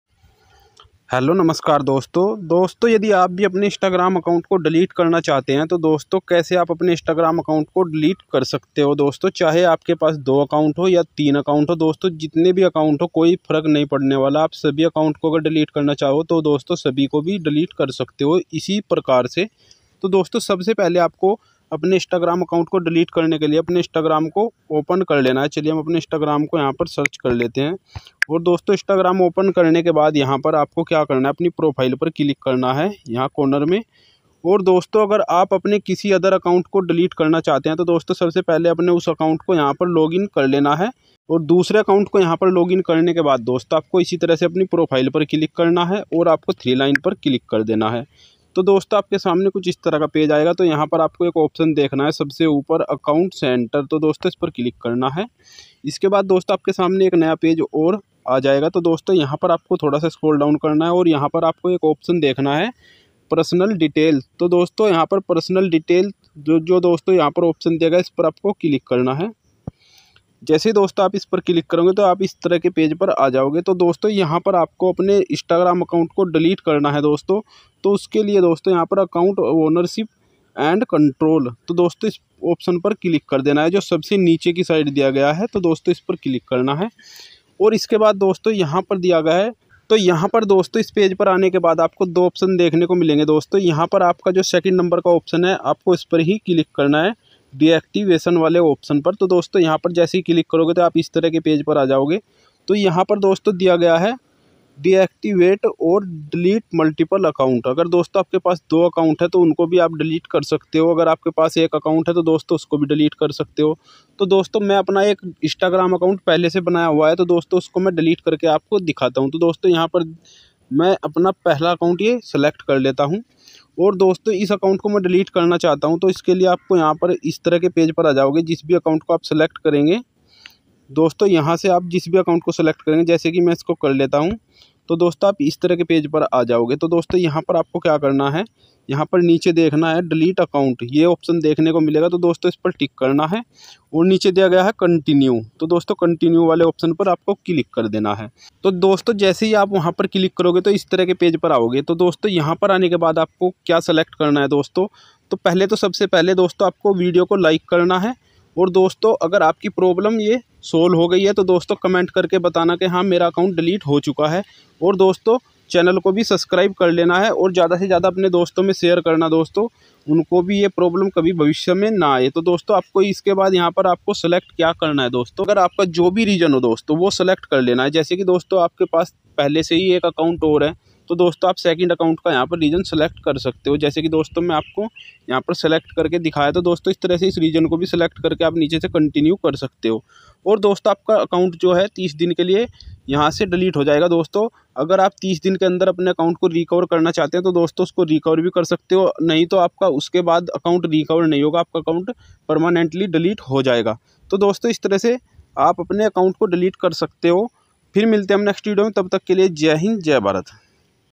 Instagram account delete kaise kare permanently
hello doston es video mein bataya gaya hai aap apne Instagram account ko permanently delete kaise kar sakte ho is video mein bataye Gaye step ko follow karke aap apne Instagram ko delete kar sakte ho